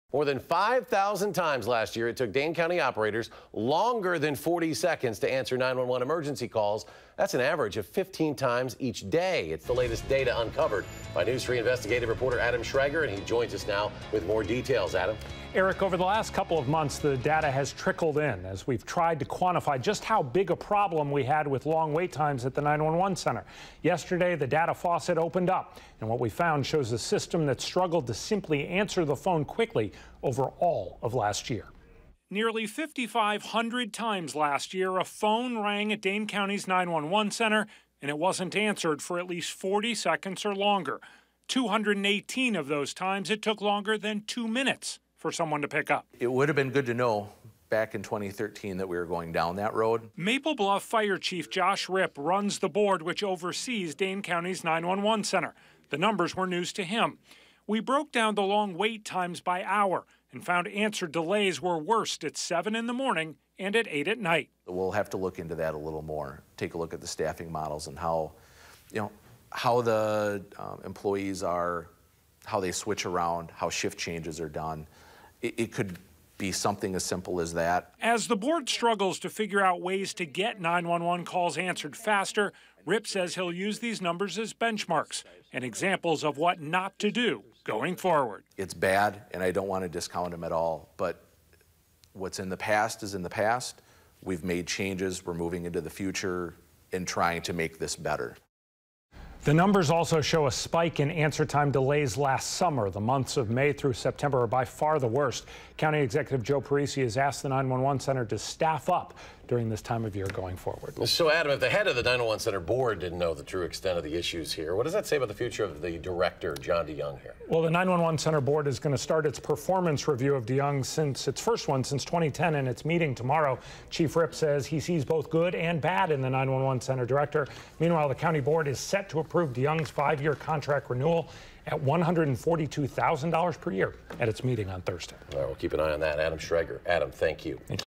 More than 5K callers wait longer than 40 seconds for 911 dispatch to answer
Emergency callers to the Dane County 911 Center waited for more than 40 seconds for an operator to answer the phone a total of 5,476 times last year. That's an average of roughly 15 times per day.